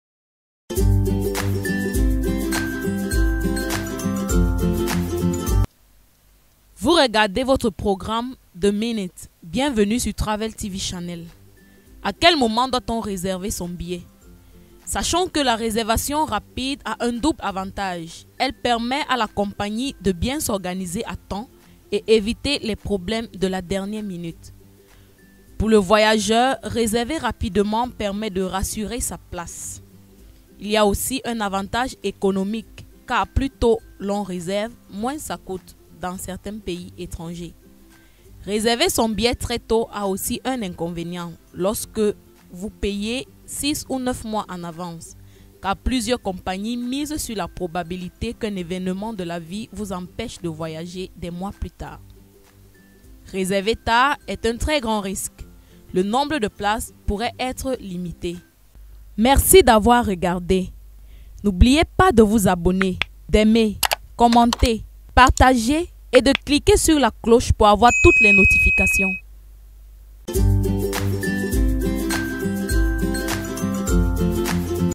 VOICI CE QUE VOUS GAGNER EN RESERVANT VOS BILLETS ASSEZ TOT
DANS CETTE VIDEO VOUS DECOUVRIREZ CE QUE VOUS GAGNEREZ EN RESERVANT VOS BILLETS DE VOYAGE ASSEZ TOT